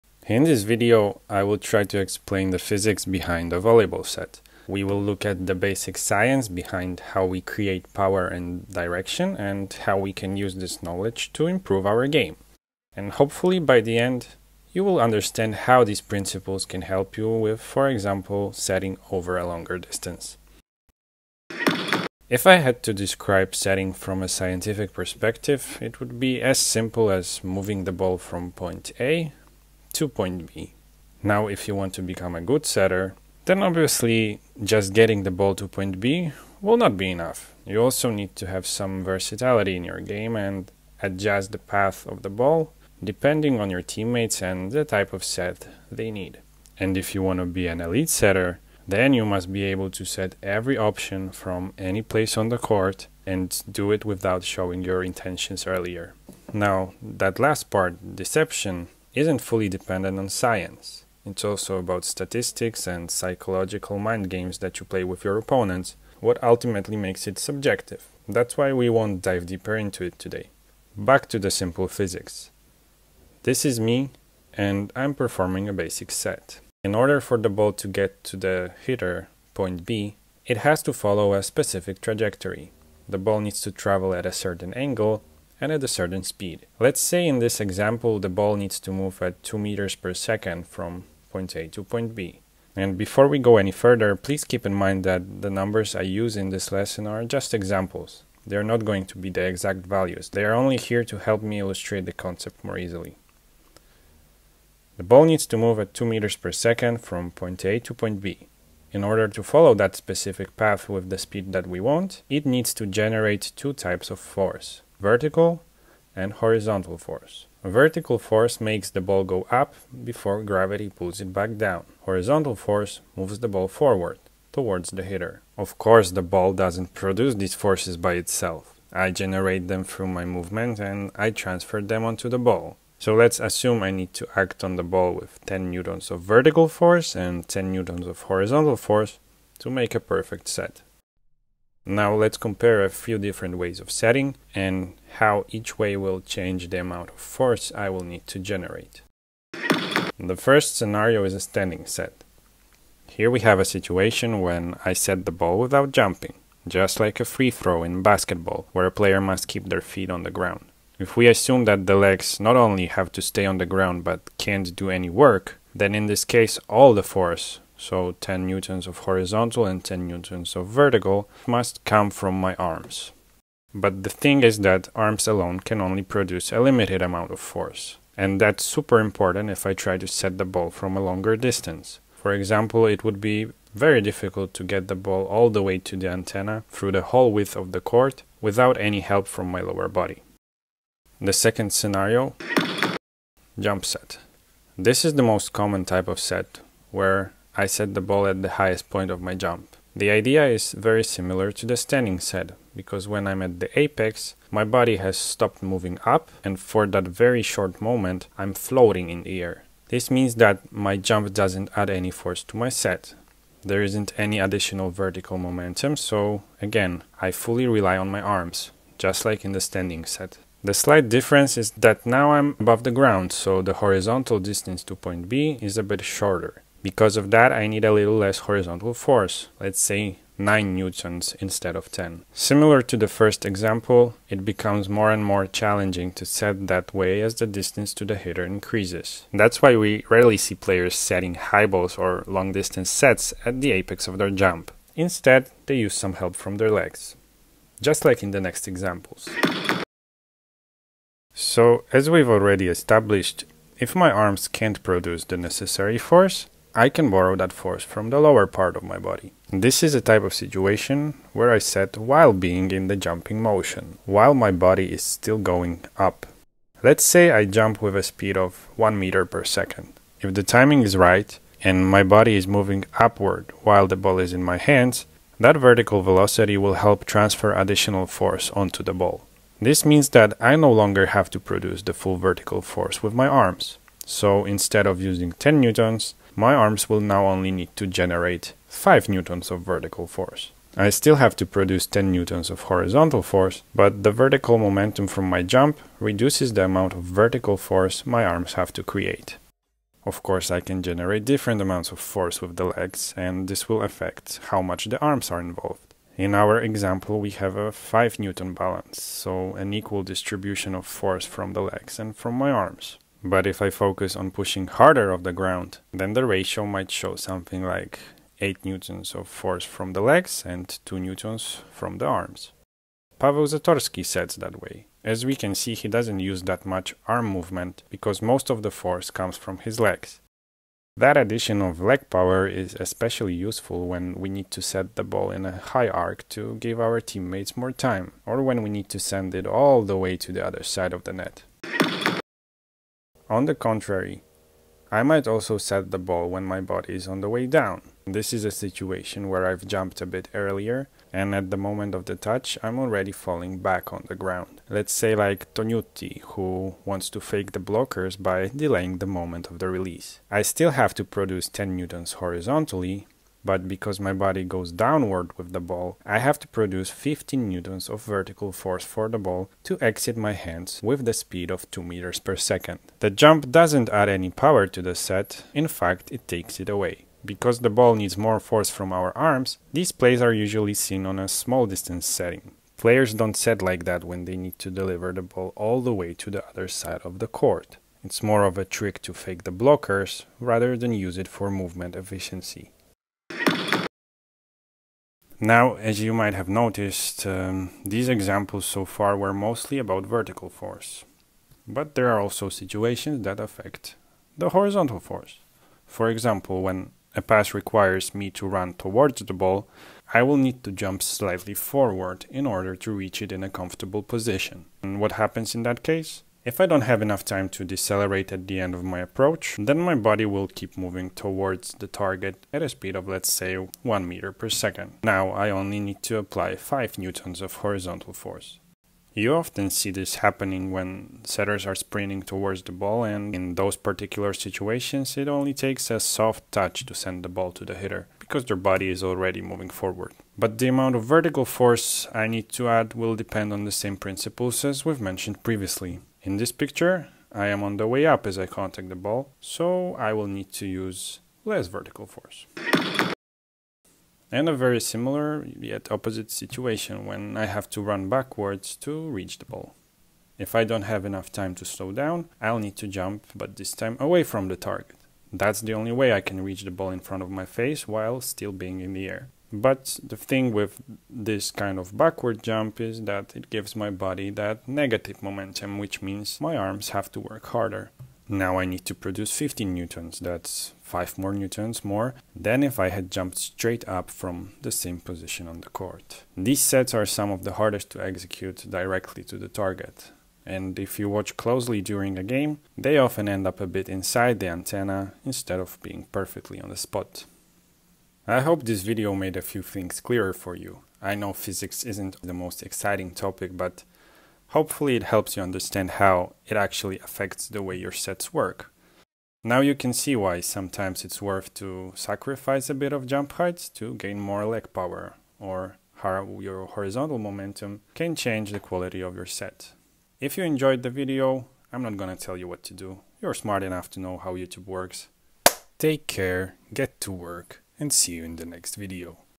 The Science of Setting: How Body Momentum Affects POWER
In this video I break down how to set with more power and how to become a better volleyball setter by focusing on the basic physics principles.

I hope it makes sense :)

0:00 - What it takes to be an Elite Setter
1:11 - The Forces Behind a Set
2:28 - Standing Set
3:11 - Jump Set
4:10 - Upward Momentum
5:42 - Downward Momentum
6:45 - Forward Momentum
7:33 - Backward Momentum
Note: Full credit to owners. All the images, pictures and the video belongs to the respected owners.